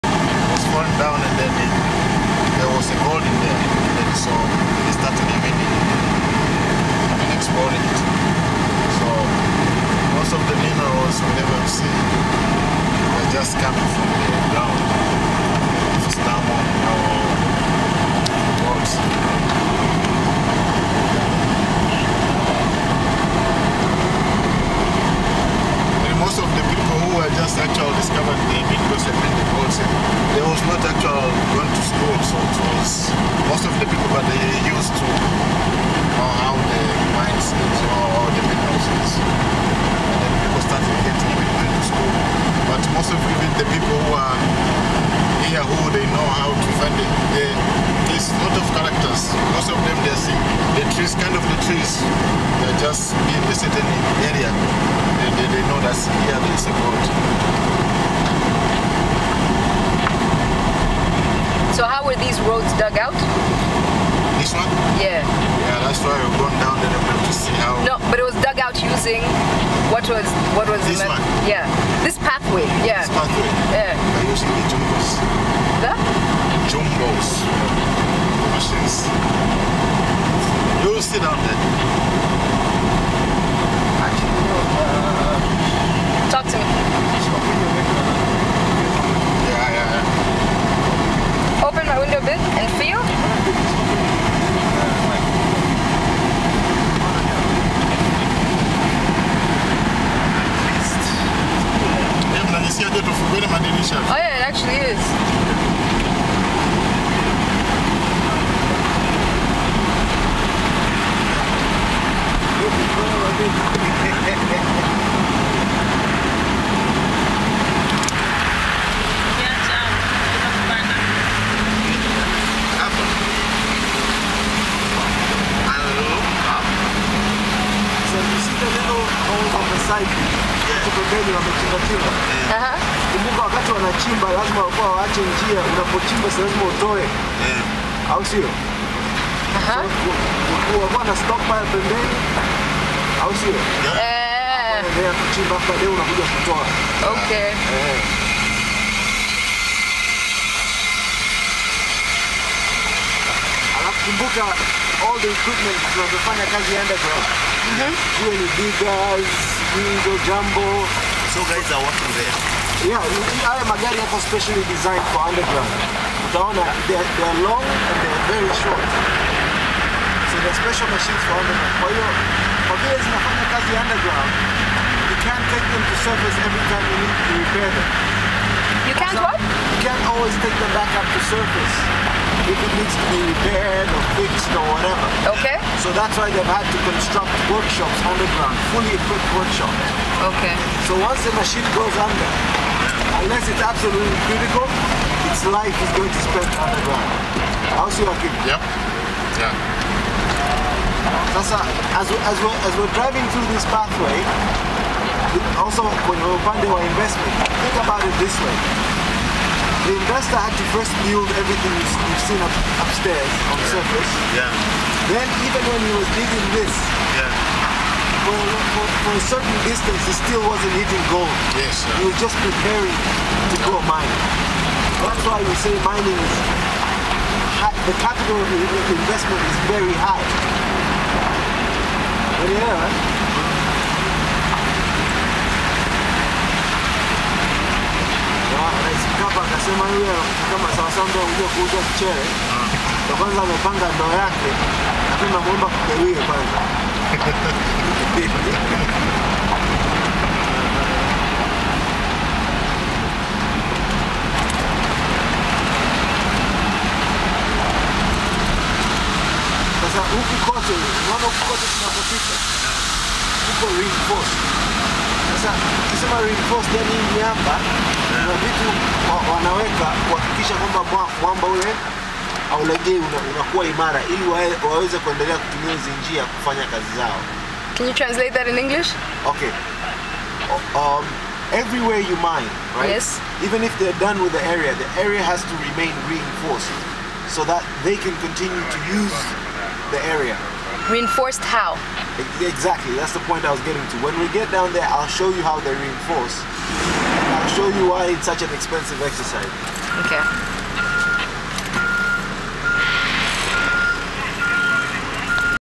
First Five Minutes Underground in Bulyanhulu Mine
This is a video I took as i was driving into African Barrick Gold's gold mine at Bulyanhulu. Sorry for the bad sound quality. If you'd like to know more about what I saw during my visit with African Barrick Gold,